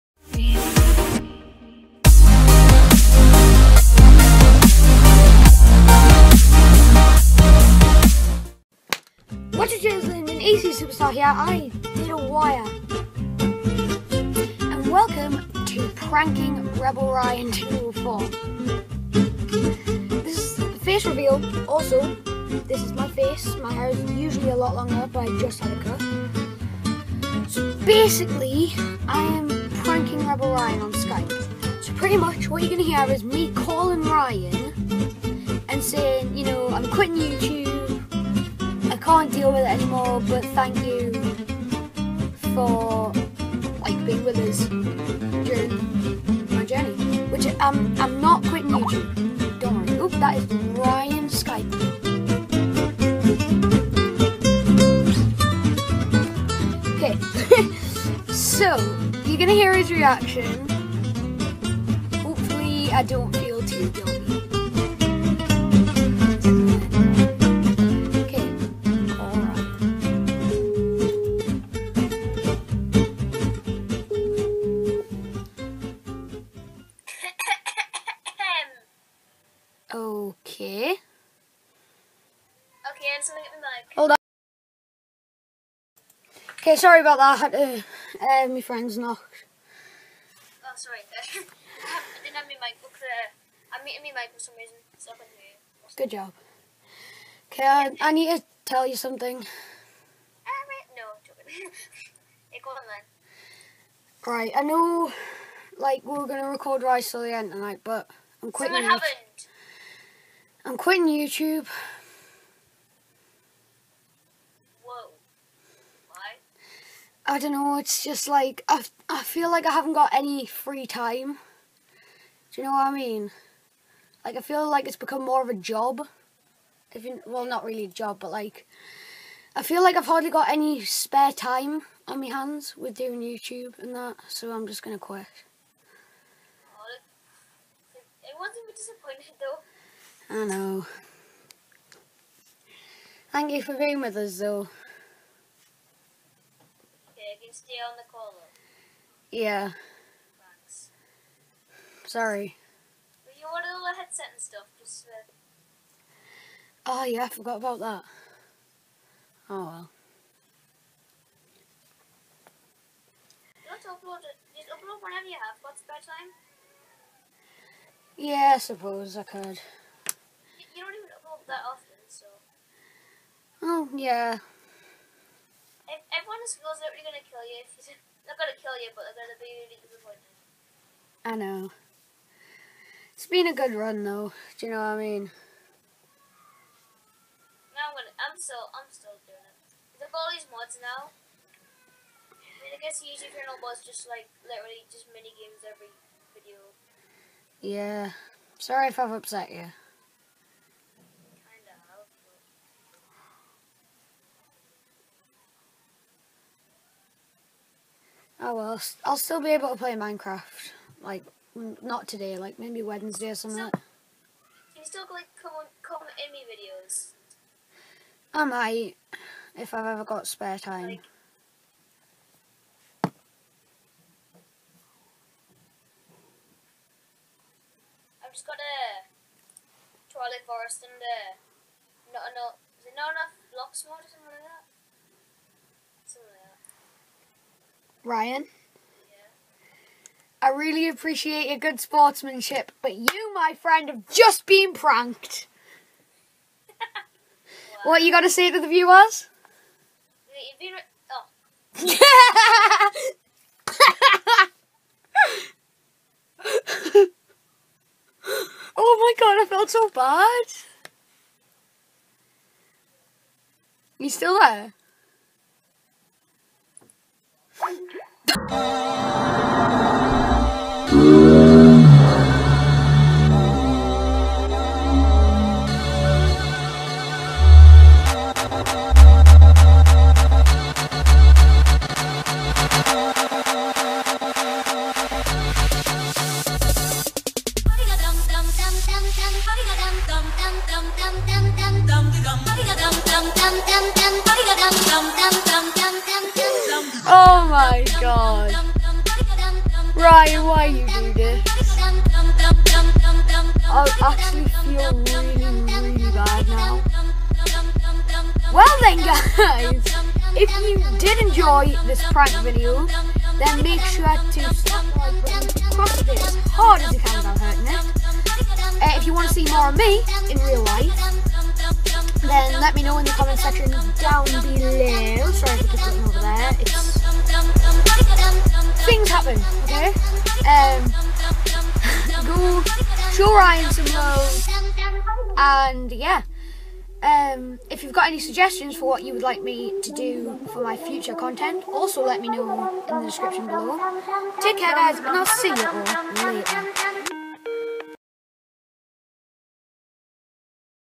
PRANKING REBELRYAN204!!!
WARNING: VIDEO MAY BE UNSTABLE DUE TO HIGH LEVELS OF CRINGE. PROCCEED WITH CAUTION

What's up Genties and Ladlemen, ECsuperstar here, and welcome to a different type of video. I decided that I would pull a little prank on rebelryan204, by telling him that I'm quitting YouTube (Which I'm not btw). He was a really good sport and took it quite well. Good job Ryan! Take care guys, and I'll see you all later